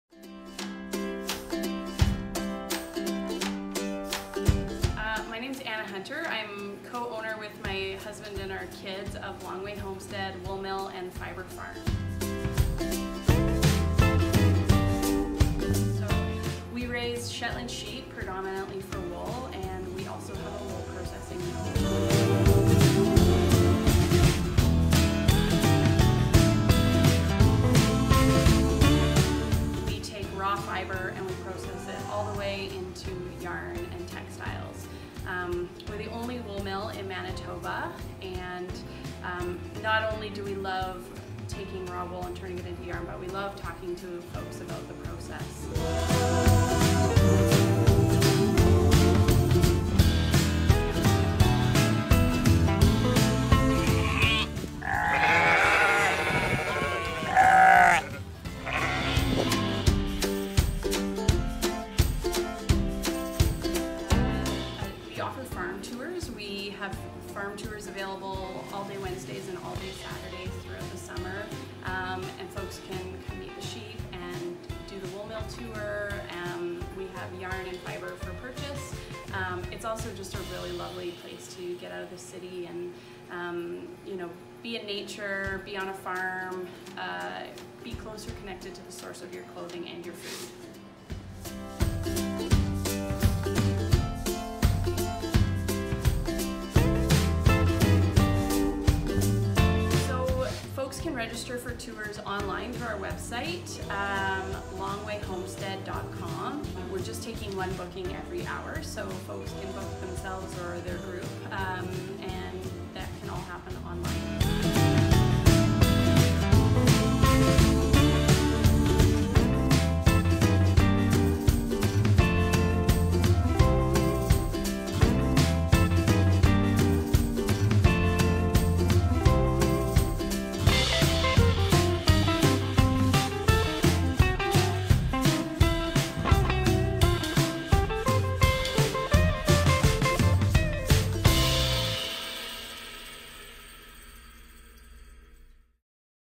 Long Way Homestead
Anna started knitting in 1999 while she was a live-in nanny in Switzerland. During a leave from her work as an anti-poverty advocate in 2009, she decided to open up a yarn store in the Hastings-Sunrise neighbourhood of East Vancouver. The store (Baaad Anna's Yarn Store) quickly became a neighbourhood staple, a great little yarn store with a selection of natural fibres and a focus on local dyers and spinners. It was also a space where community thrived - knitters or not! After five and a half years in business it was time for a new challenge, and the store was sold to a long-time friend and customer and the store still operates in East Van. Anna has always had a passion for building community, challenging injustice, and creating alternatives. Long Way Homestead is the next step in pursuing these passions, as she seeks to connect people with local food and clothing production.